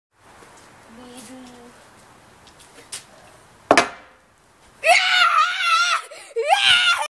AMAZING Basketball Hoop Bottle Flip Upside down Landing
Best bottle flip - amazing basketball Hoop landing upside down crazy